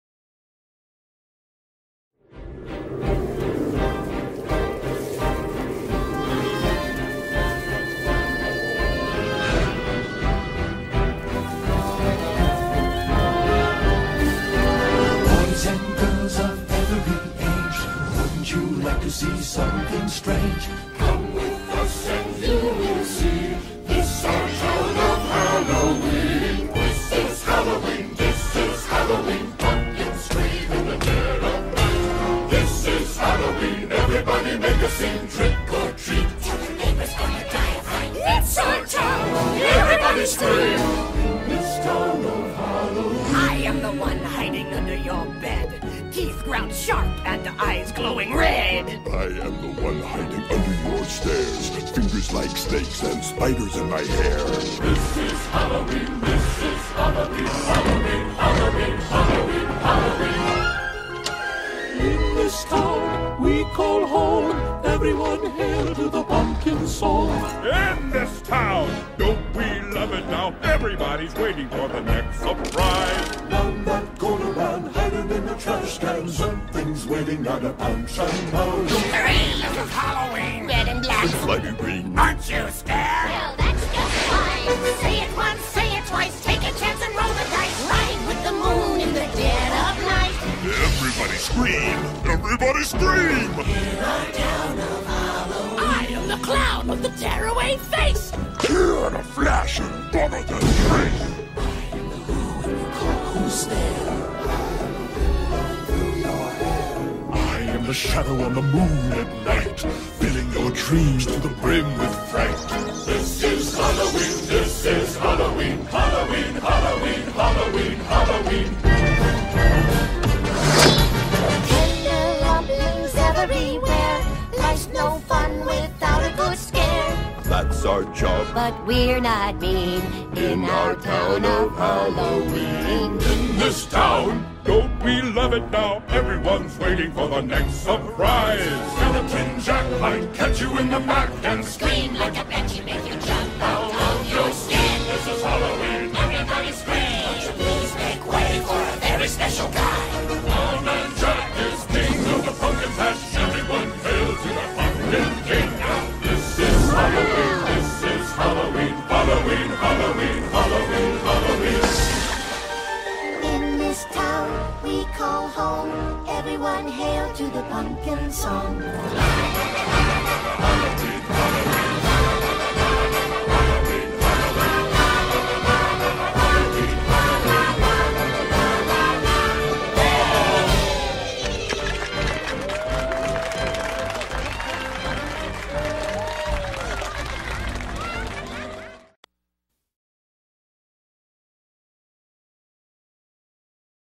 Dalton and friends, Halloween poster special
hope you guys have a spooky Halloween, I really love how this poster turned out and the characters let me know what you think